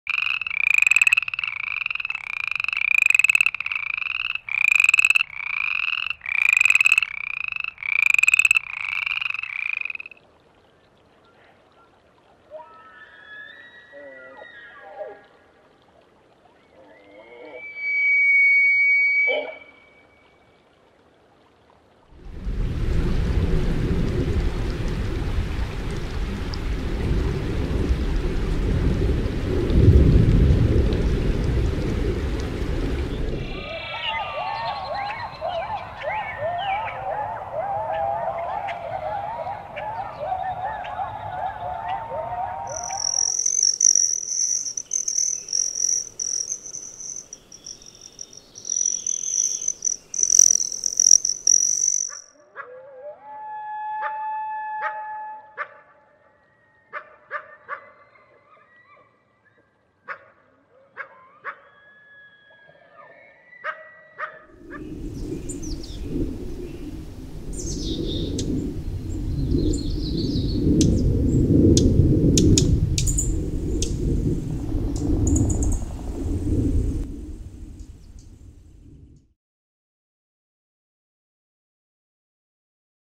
Listen to the sounds of Rocky Mountain National Park from home
In celebration of World Sound Day, RMNP released its Rocky Mountain National Park Sound Library which includes audio files of more than 200 natural sounds that occur in the park.

From the calm sounds of rushing water to elk bugles, birds chirping and coyotes calling, you can listen to it all from the comfort of your home or office.

All the sounds were collected in the park by Dr. Jacob Job, a researcher with Colorado State University and the NPS Natural Sounds & Night Skies Division.

The Sound Library is part of the ongoing commitment of the National Park Service to document and preserve the natural sounds of the environment.